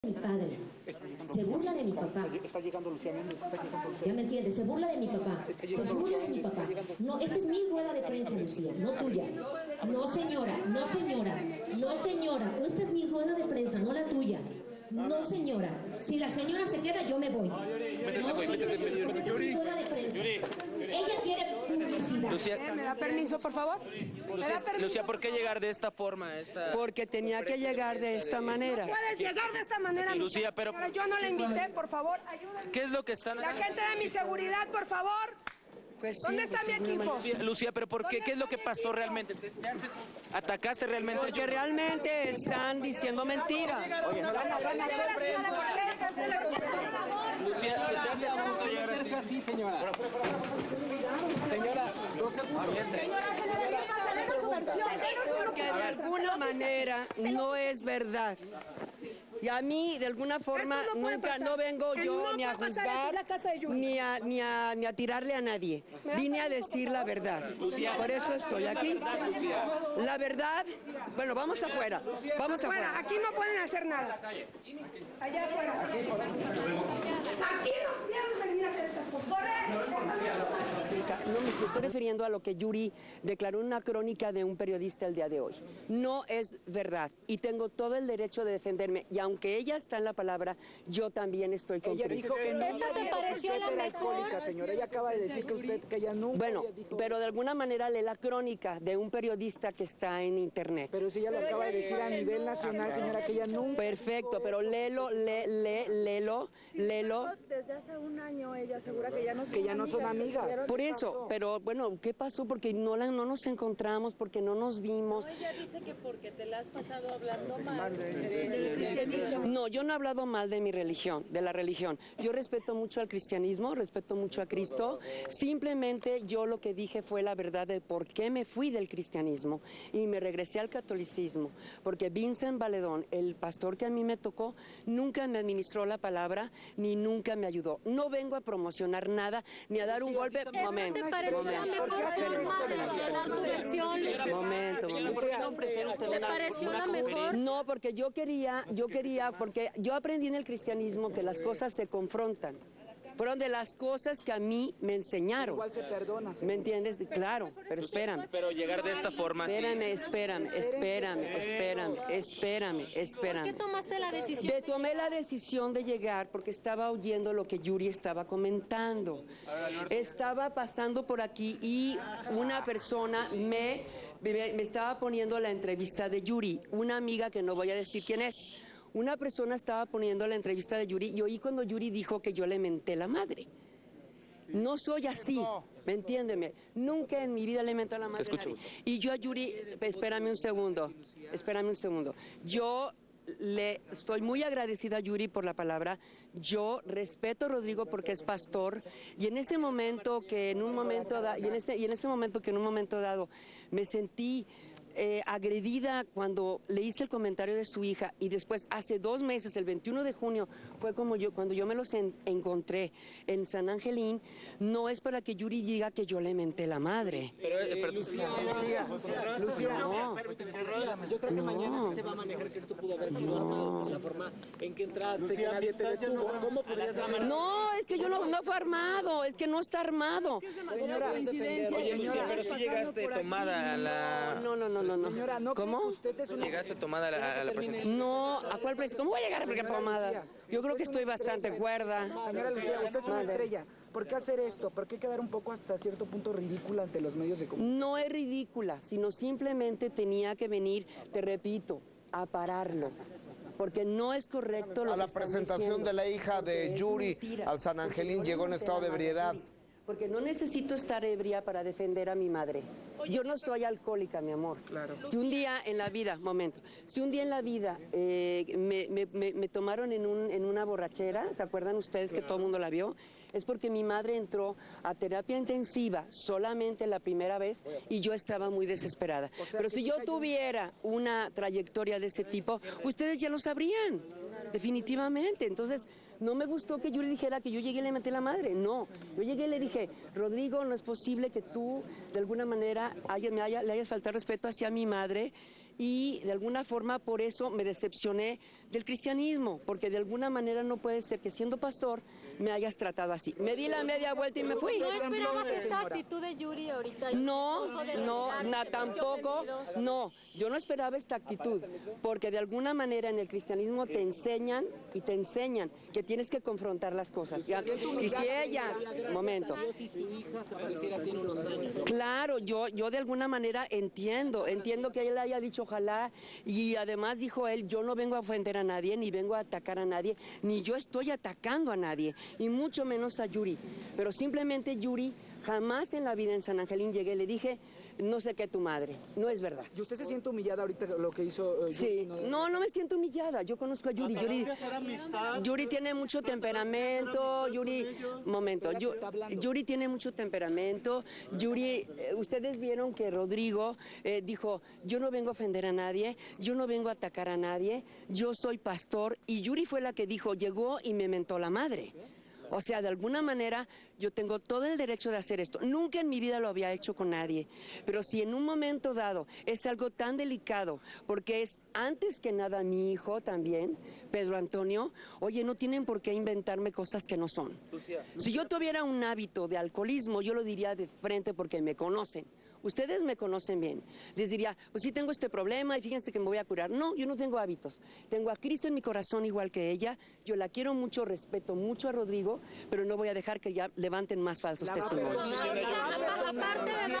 LUCIA MENDEZ EN LA CONFERENCIA DE PRENSA DE YURI
LUCIA MENDEZ ACUDIÓ A  LA CONFERENCIA DE PRENSA DE YURI PARA ENFRENTARLA POR  LAS DECLARACIONES QUE YURI ESTABA HACIENDO.